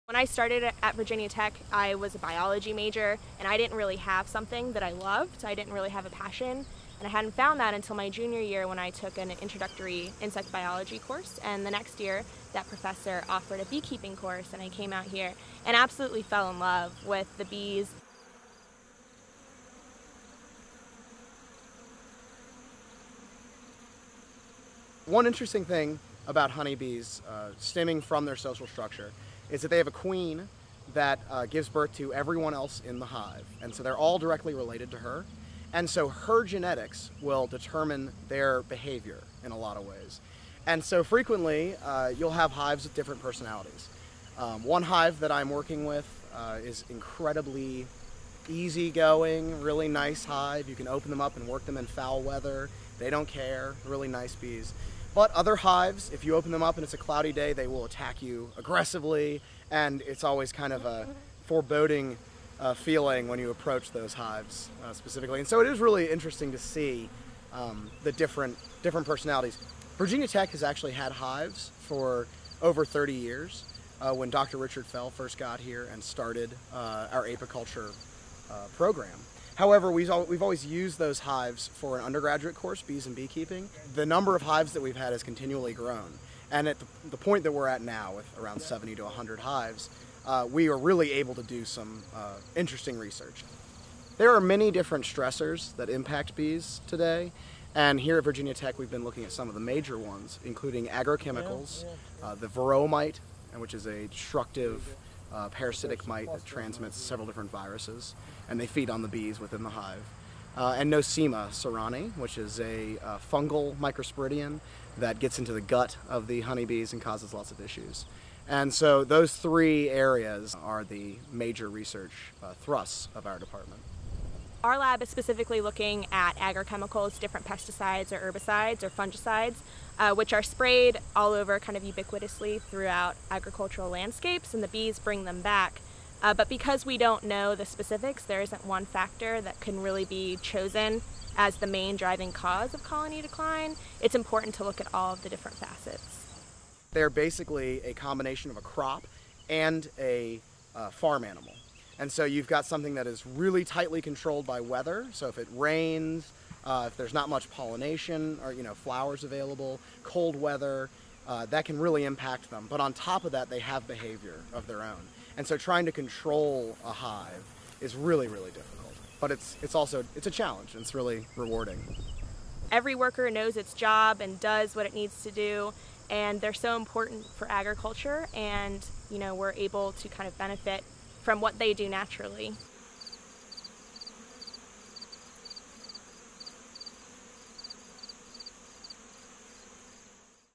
Working with Bees - Virginia Tech
Working with bees can be challenging because each hive has a different personality, but for some researchers in the Department of Entomology that's just part of what makes their work rewarding.  Virginia Tech has over 70 hives which are used in studies on agrochemicals, varroa mites and nosema ceranae parasites.